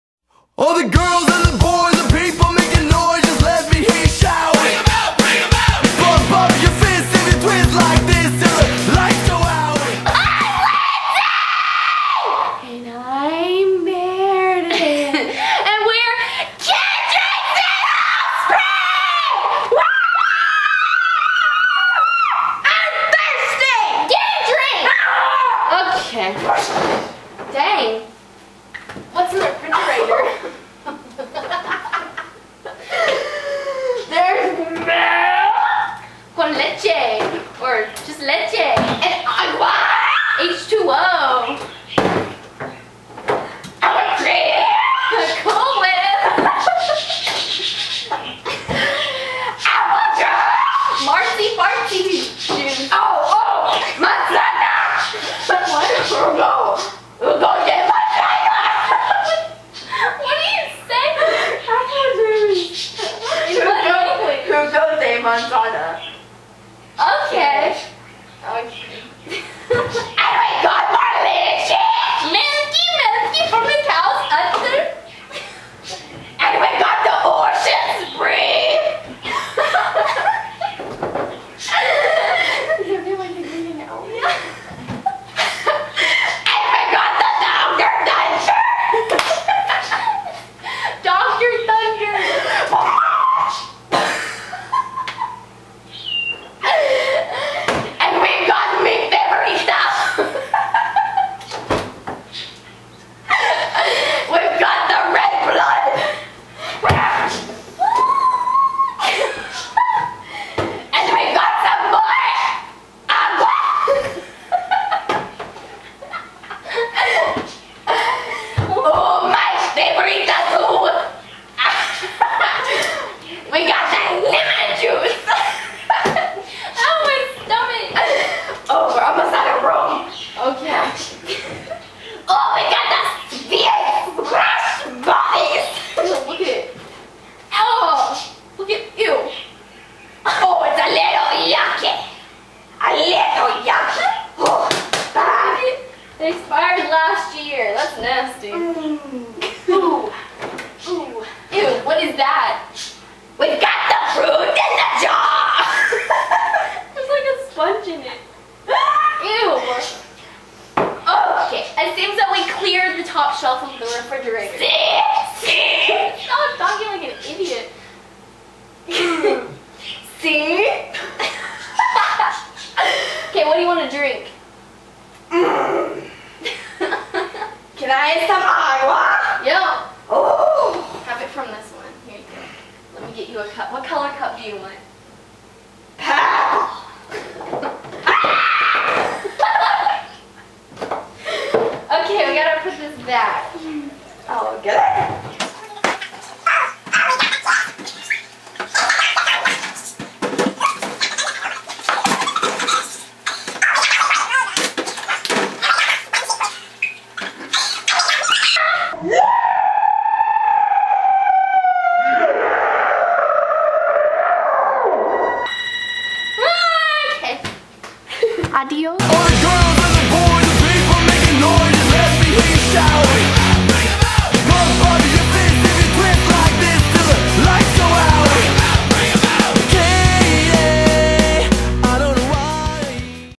Kendrex And Offspring Raid the Fridge!
yah.. offspring was a little thirsty i guess!